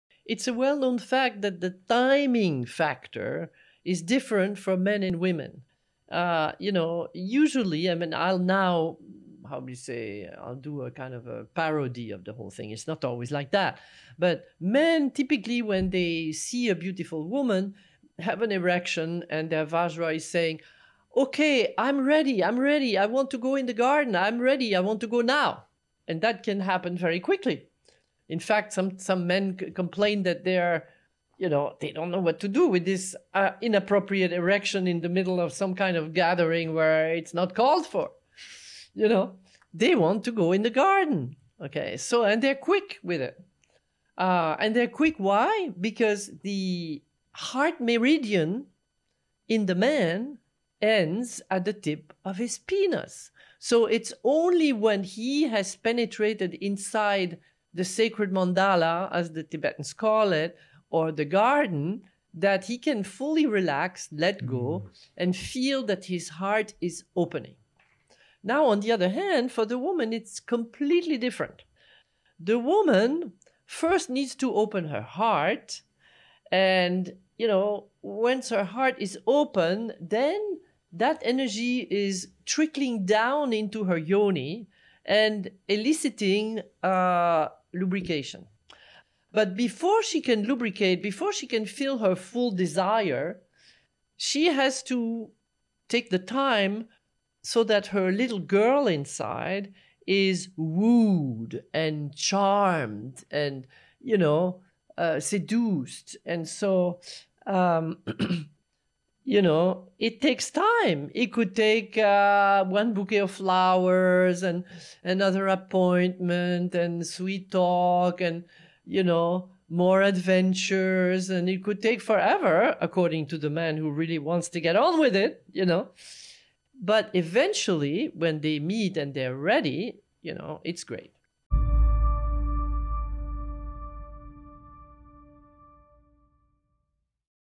The Different Sexual Timing of Men and Women - Margot Anand
Margot talks about the timing factor between men and women when it comes to being aroused in sex. She passionately explains how the way for a woman to be stimulated into sex and that it is an amazing thing when the arousing of men and women meet and are both ready. (You can connect with us

Here are the great points we covered during this interview:
[7:03] The relationship between Sex and Spirituality
[11:00] The Importance of Moving the Sexual Energy Upwards
[16:01] The Different Sexual Timing of Men and Women
[19:33] Different Dimensions To Bliss
[40:18] Are Psychedelics a Shortcut to Enlightenment

Join the conversation by COMMENTING below.